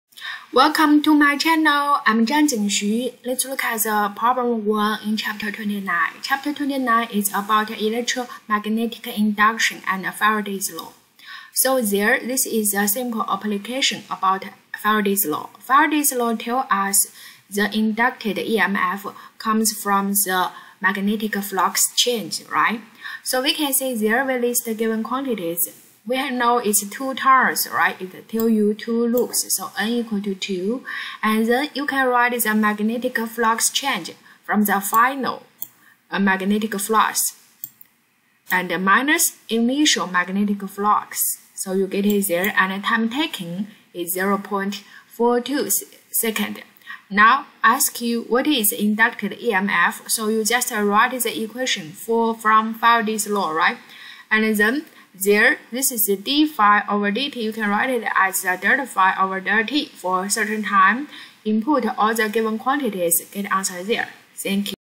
(29-1) The magnetic flux through a coil of wire containing two loops changes at a constant rate from
(29-1) The magnetic flux through a coil of wire containing two loops changes at a constant rate from - 58 Wb to +38 Wb in 0.42 s. What is the emf induced in the coil?